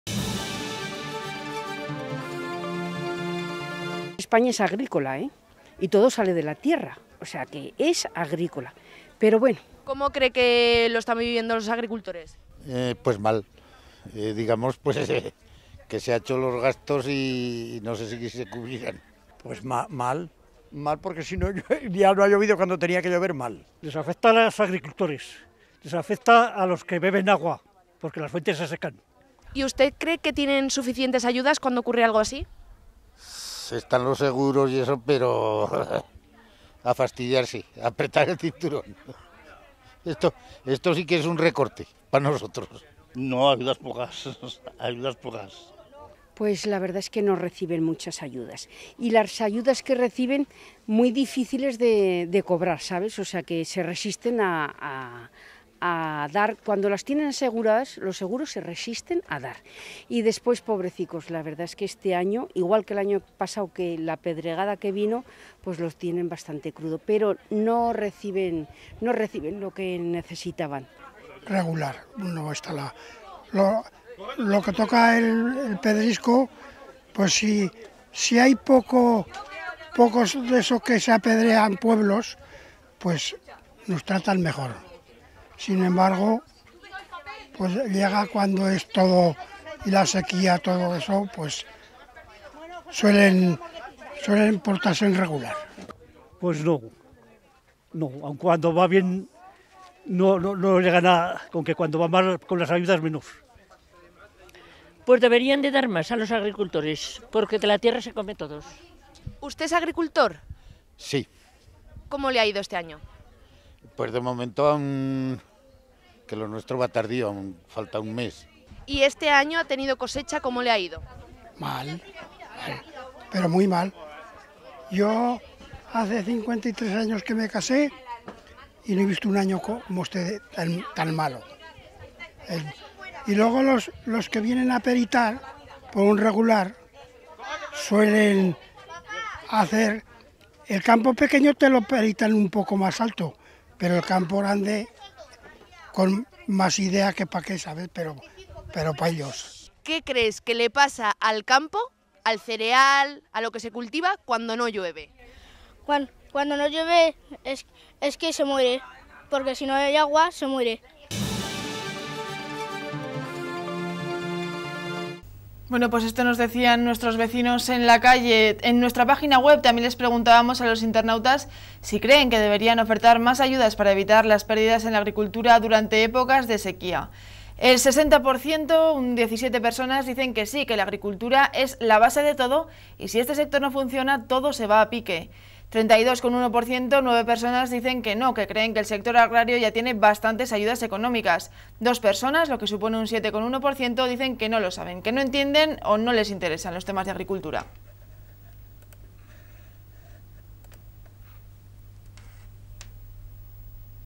Encuesta: ¿Crees que habría que establecer ayudas por la sequía?
La sequía ha marcado la campaña agrícola. Muchos vecinos creen que hay que compensar las pérdidas que ocasionan.